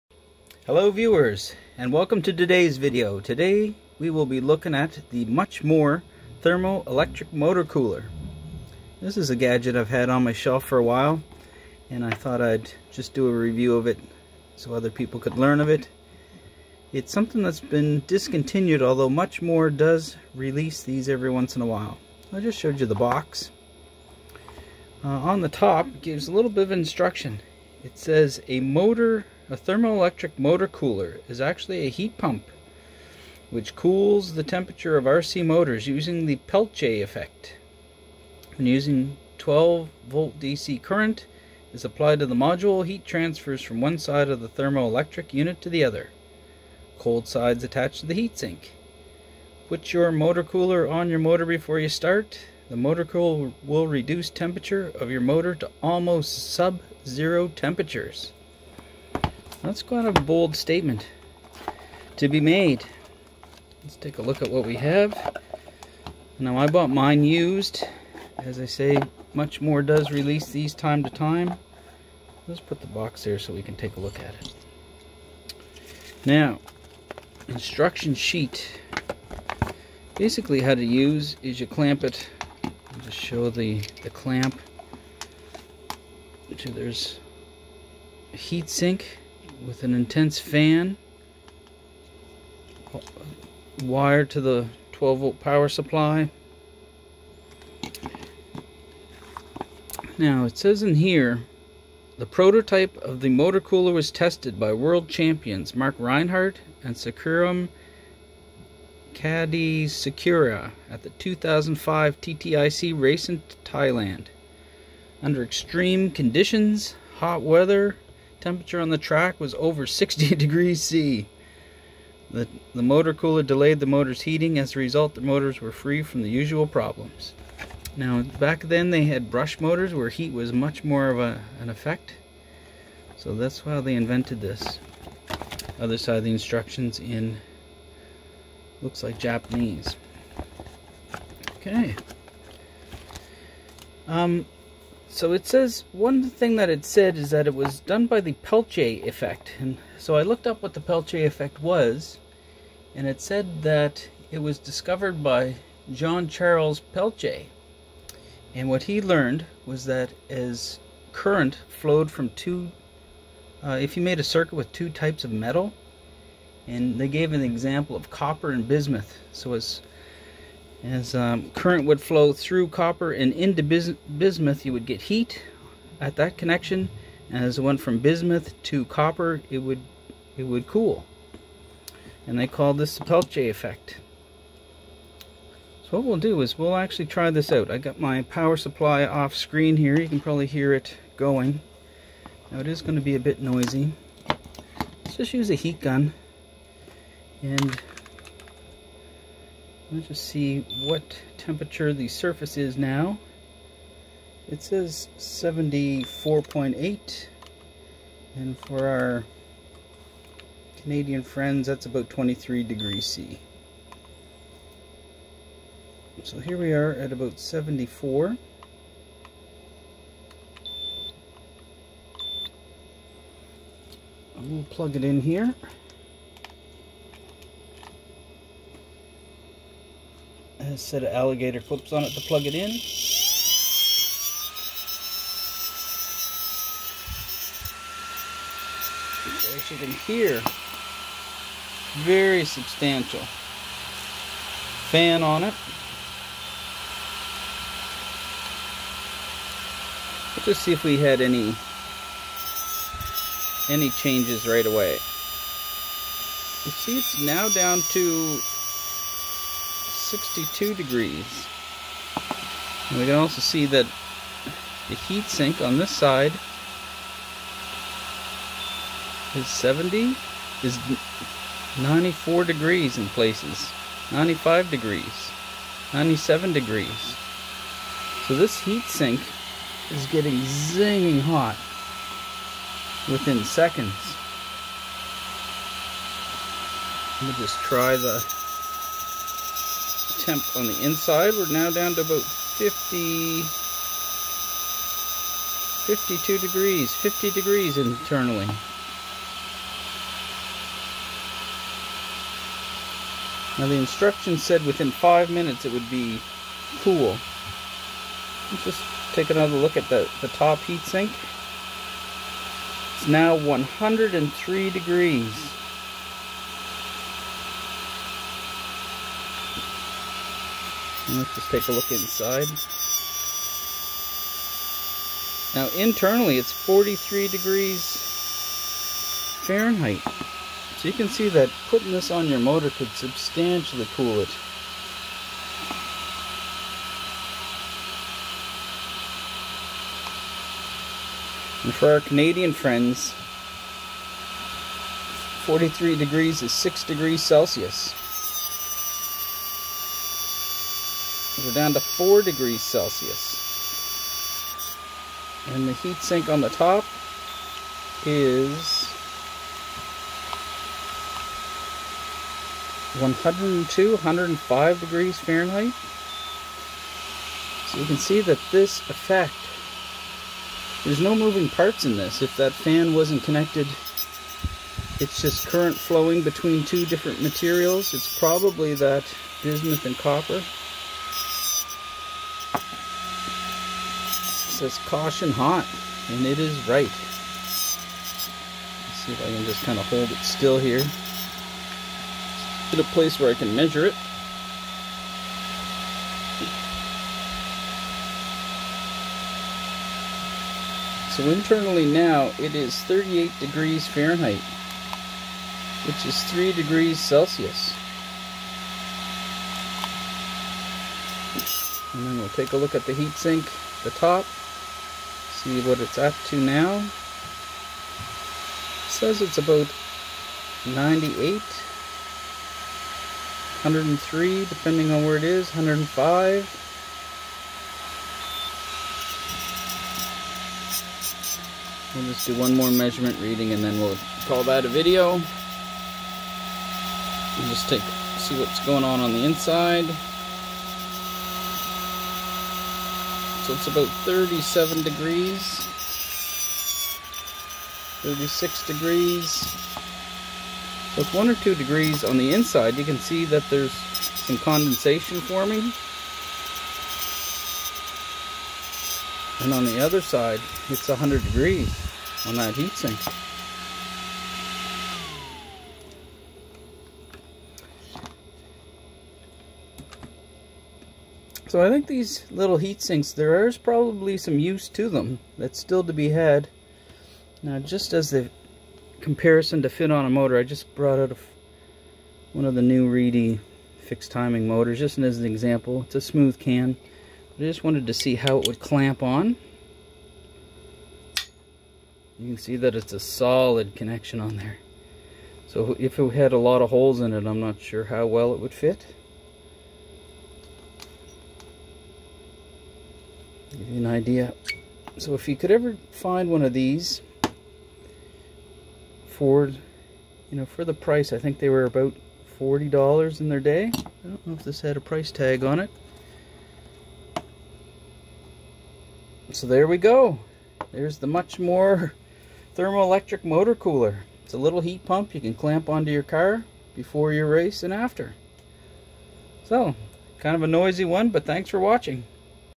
Review: Much More Thermo-Electric Motor Cooler: MR-TMC
This is a review of my Much More Racing Thermo-electric Motor Cooler, model: MR-TMC.

This is a sweet little motor cooler that can cool the stator of the motor in minutes. It is designed to be used to cool the motor before the race, ROAR rules would prevent it from being run in a car during race time. This may be good for the speed run guys who pull a ton of amps and heat up equipment during their runs.

From the description:
A Thermo-Electric motor cooler is actually a heat pump which cools the temperature of R/C Motors using a Peltier effect.

When a 12V DC current is applied to the module, heat transfers from one side of the thermoelectric module to the other. The cold side is attached to the heat sink. Put your TMC on your motor before you start, the TMC will reduce the temperature of your motor almost to sub-zero temperatures.

The prototype of TMC was tested by World champions - Marc Rheinard and Surikarn Chaidajsuriya at the 2005 TITC race in Thailand. Under extremely hot weather condition (the temperature of the track surface was over 60deg.C) the TMC delayed their motor's heating. As a result, their motors were free from the usual problems.